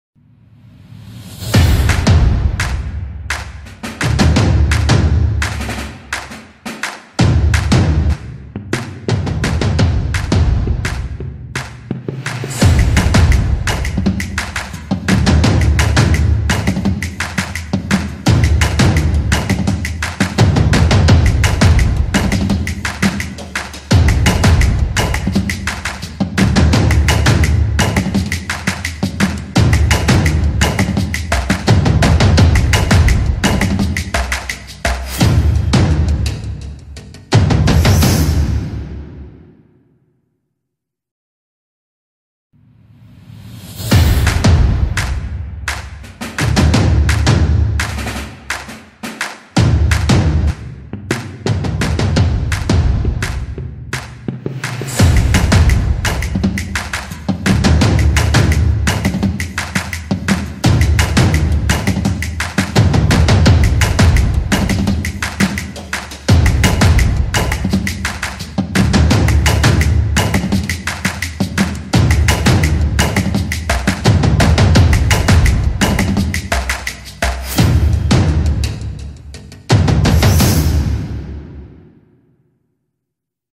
Stomp Opener - After Effects Project Files | VideoHive 19906838
Description This beautiful and simple After Effects template features 31 placeholders for your images or videos, 38... (openers)